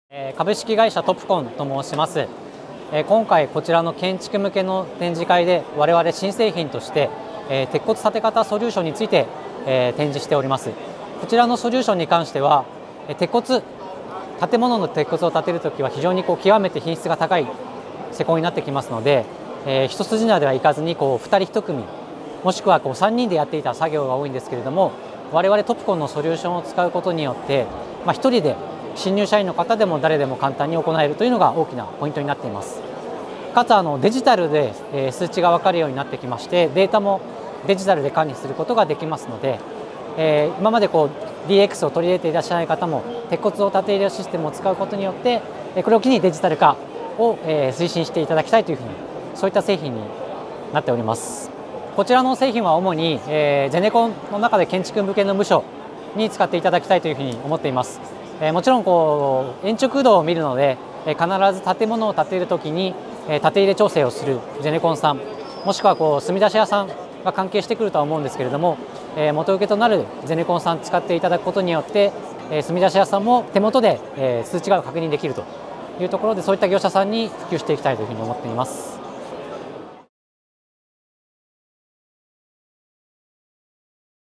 [第8回 JAPAN BUILD TOKYO－建築の先端技術展－] 鉄骨建方誘導ソリューション - 株式会社トプコン
株式会社トプコンは第8回 JAPAN BUILD TOKYO－建築の先端技術展－にて、鉄骨建方誘導ソリューションを出展。